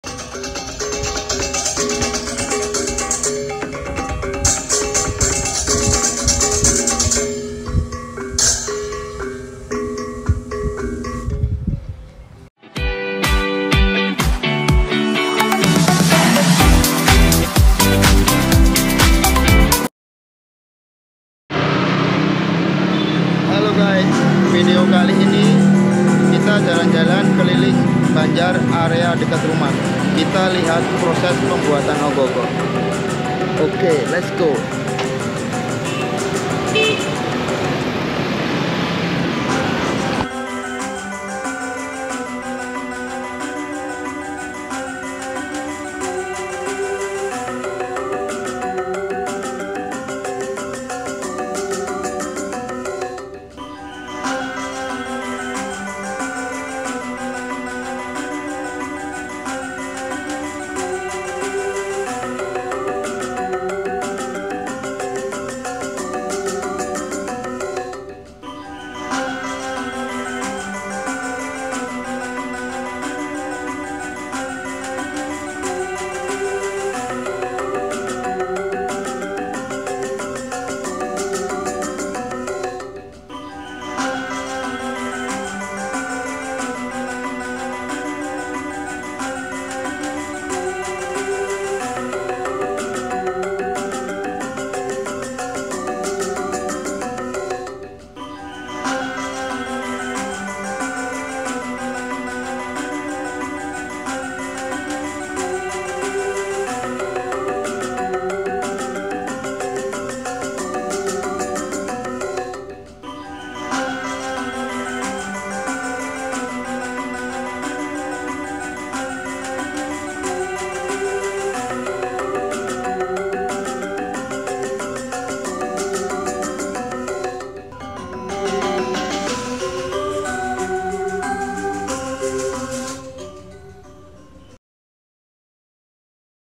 OGOH OGOH 2024 | Persiapan Nyepi 2024
Ogoh ogoh merupakan Budaya Sodara kita Umat Hindu di Indonesia.Untuk Pulau Bali Sendiri Sangat Istimewa karena di Lombakan.
Dan Setiap Pemenang nya akan di arak di Pusat kota Denpasar tepat nya di Patung CATUR MUKA.diadakan setiap tahun menjelang hari Raya Nyepi.

Terima Kasih.